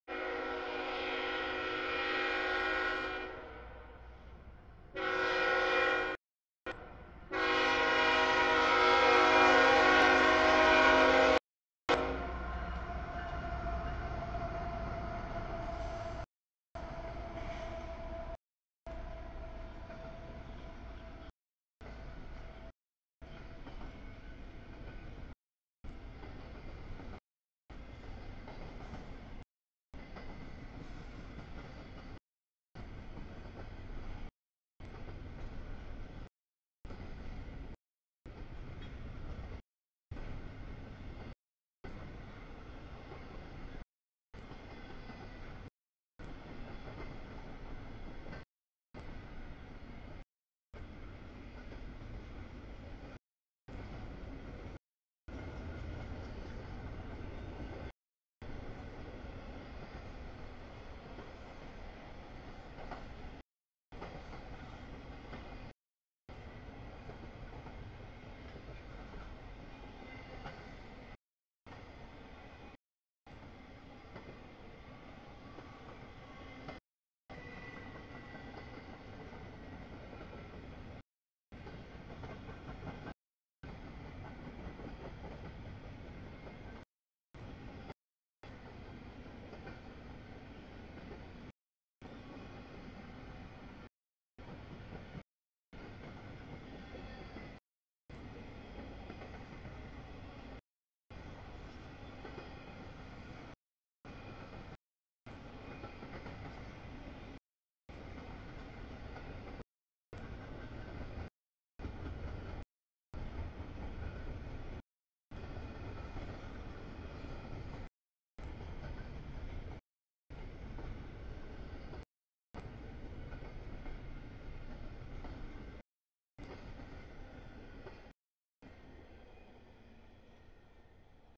1- 1-2019 07:07 nb stack CSX 3251 Q003
00:56 nb stack trailers CSX Q009
01:20 nb stack CSX 222 Q157
02:08 sb tanks CSX 5433, 71 K682
05:10 sb stack trailers CSX Q
05:58 sb mix CSX 547 Q410
07:07 nb stack CSX 3251 Q003
07:50 sb stack CSX 521, 569 L008
07:57 nb empty's BNSF 8072, 4010, 9874 CSX 5318 K163
08:36 sb stack CSX 5485, 5338 Q158
08:40 nb empty's BNSF 6923, War Bonnet 676, 5921 K139
09:27 nb mix CSX 154, 7882, 755, 495 Q190
12:19 nb mix, blue trash CSX 7777, 5499 Q434
13:01 sb green trash, mix CSX 5273,7885 Q439